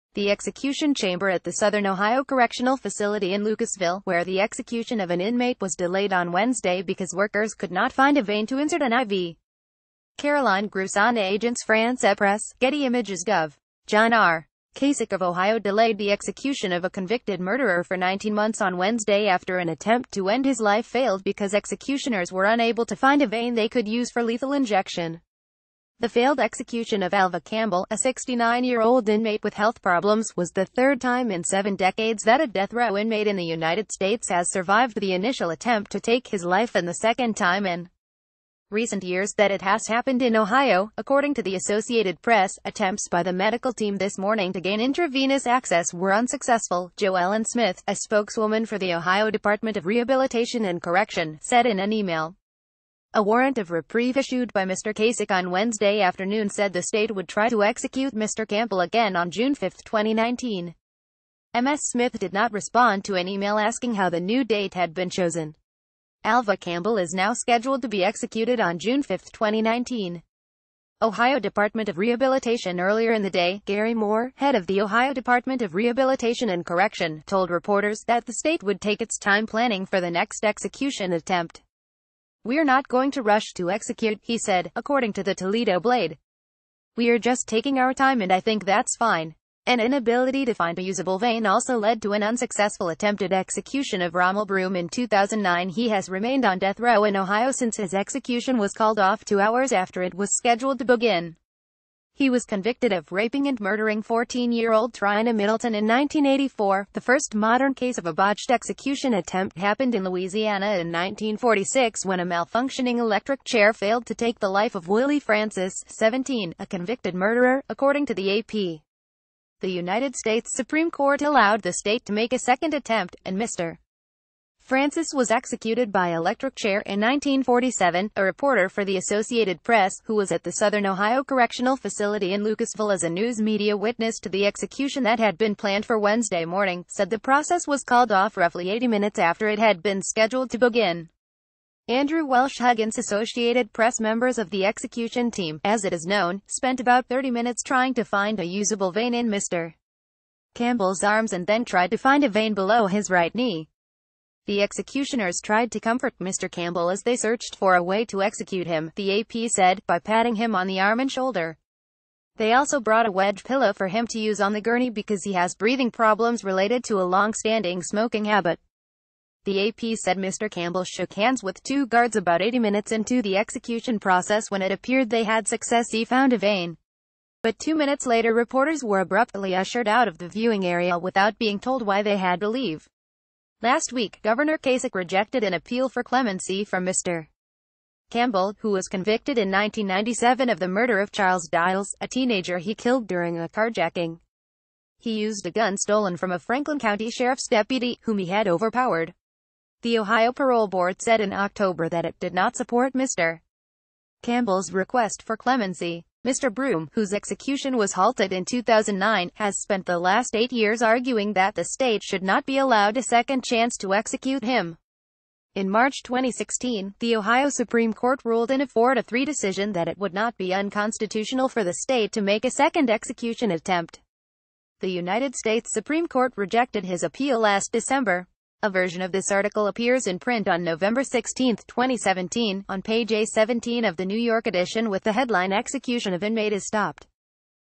AMERICAN NEWS TODAY - Made in ohio stopped after not having varicose veins can use can be found
AMERICAN NEWS TODAY - AMERICAN NEWS TODAY - Made in ohio stopped after not having varicose veins can use can be found

The execution chamber at the Southern Ohio Correctional Facility in Lucasville, where the execution of an inmate was delayed on Wednesday because workers could not find a vein to insert an IV. Caroline GroussainAgence FrancePresse — Getty Images Gov. John R. Kasich of Ohio delayed the execution of a convicted murderer for 19 months on Wednesday after an attempt to end his life failed because executioners were unable to find a vein they could use for lethal injection. The failed execution of Alva...